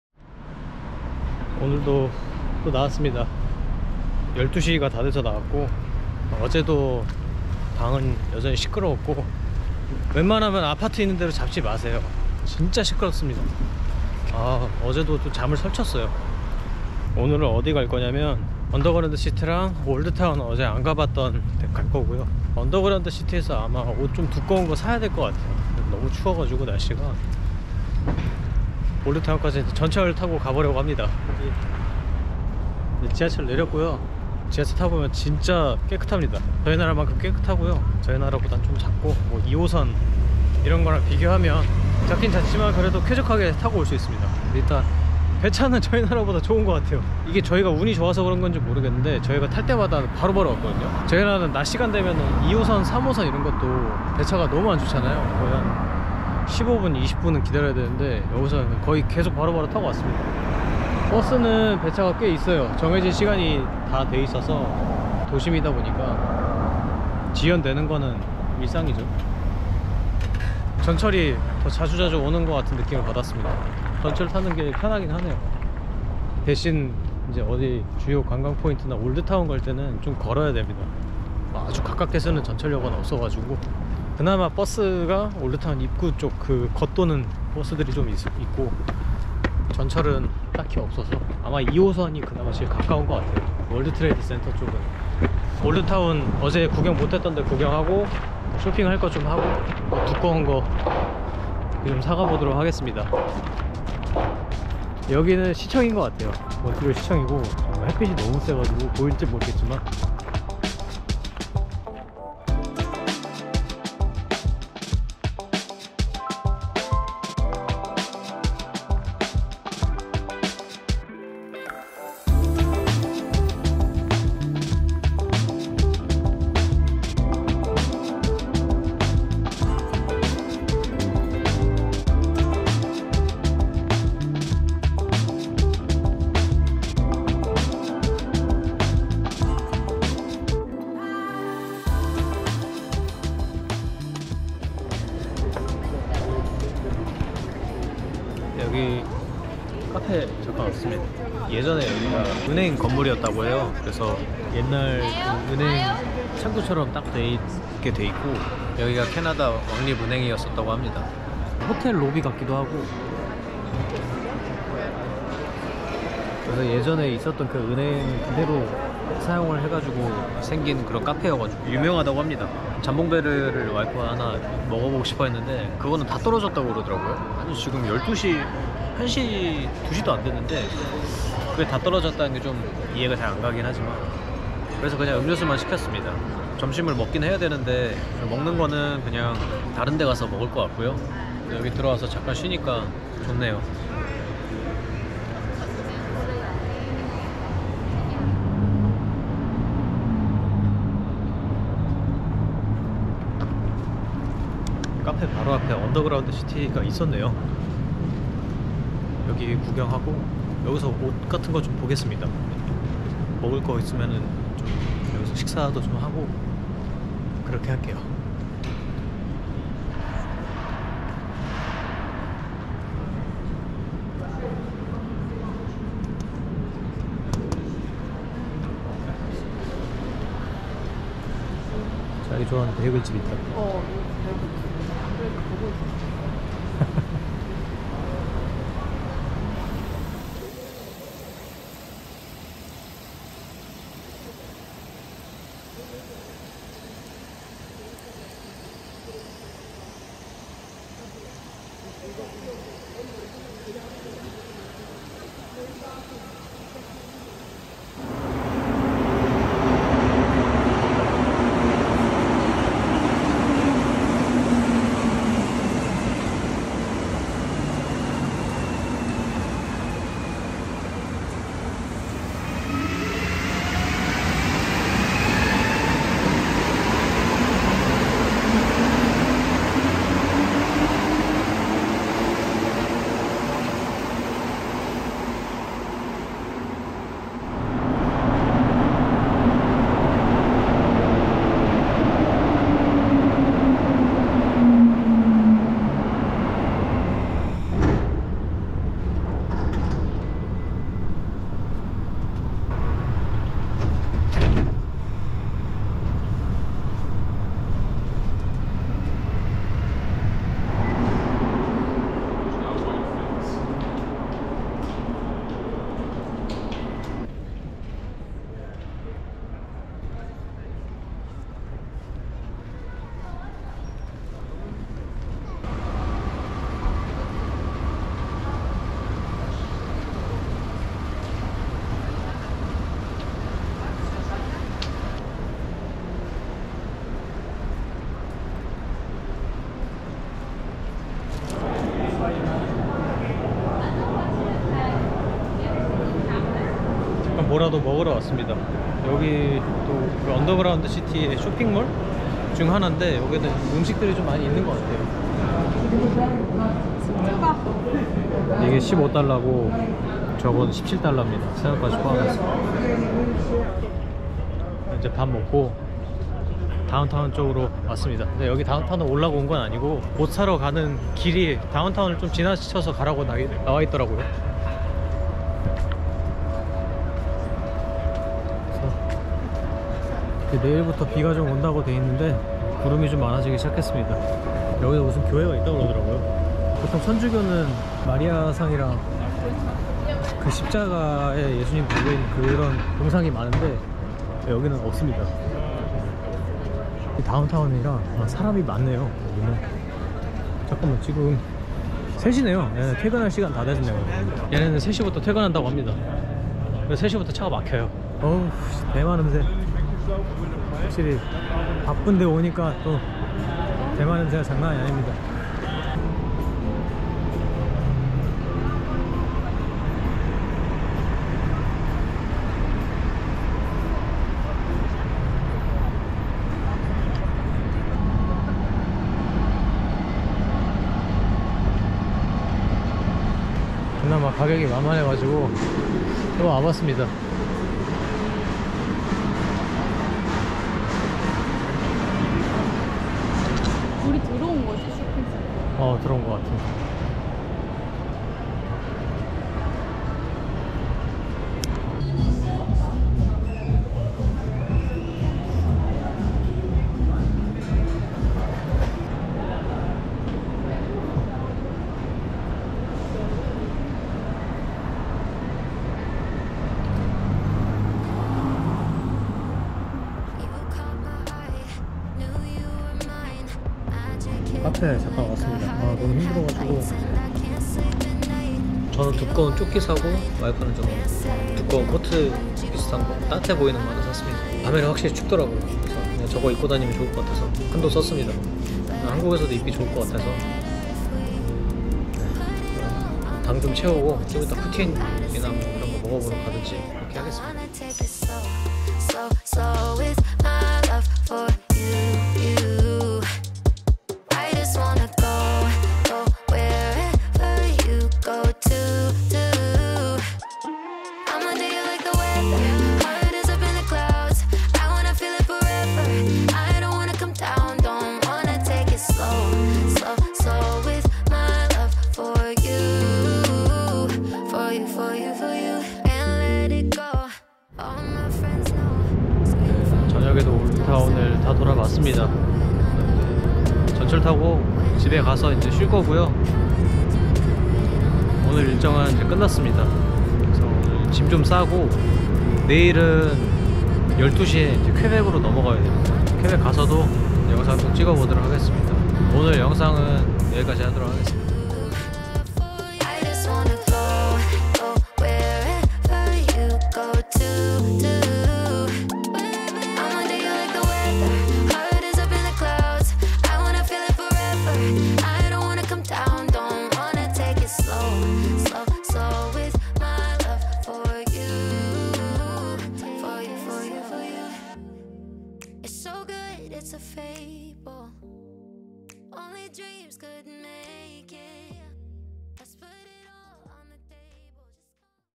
[로드트립-44🇨🇦] 충분히 둘러본 몬트리올
마지막 몬트리올 입니다.
팟타이는 정말 최악이었습니다.

[BGM]
유튜브 유료 음원 사이트 Artlist
위 URL로 가입하시면 2개월 무료로 추가받으실 수 있고 저도 2개월 연장 혜택을 받습니다